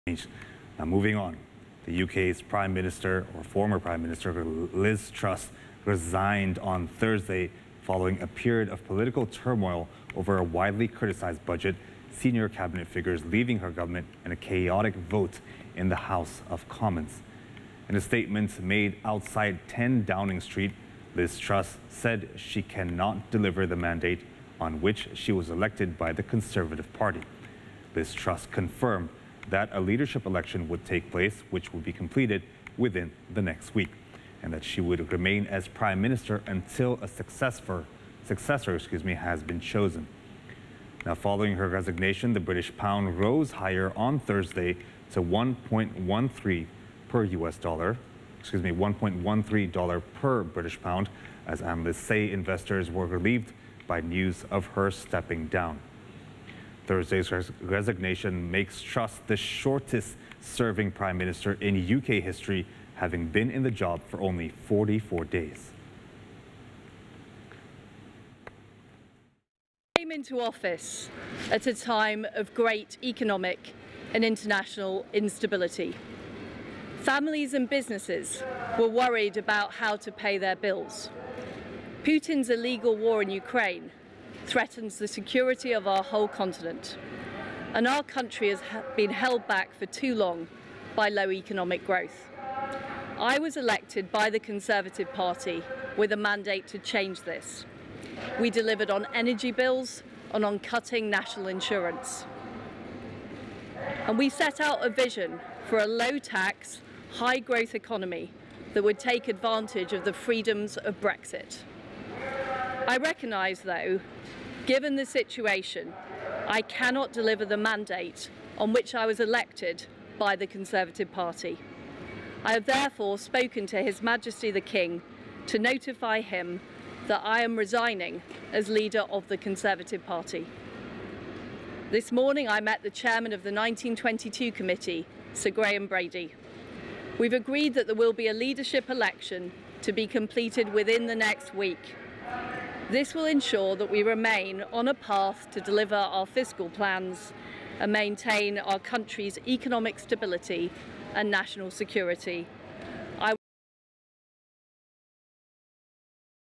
Liz Truss Resigns As U.K Prime Minister
U.K Prime Minister Liz Truss resigned on Thursday following a period of political turmoil over a widely-criticized budget, senior cabinet figures leaving her governments and a chaotic vote in the House of Commons.